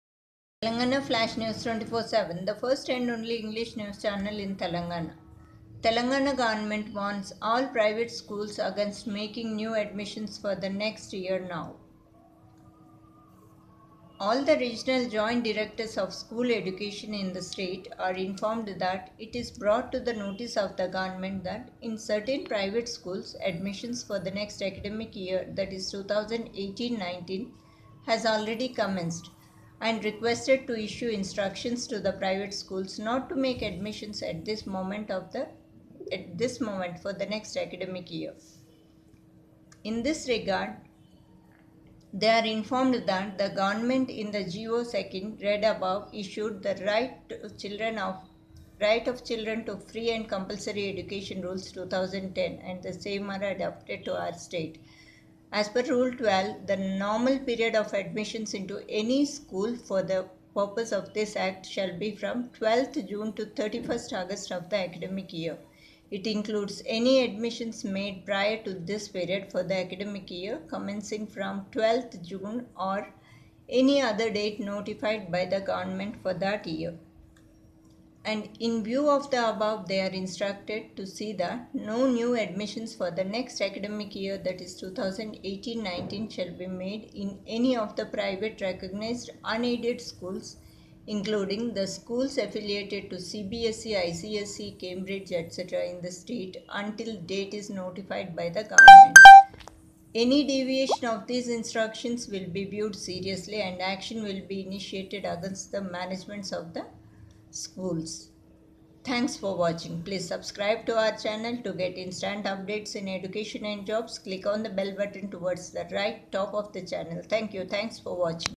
Private Schools Admissions in Telangana 2018: No Admissions for Next Year (2018-19) Now: Govt order
Telangana Government  warns all private schools against making new admissions for next year (2018-19) now.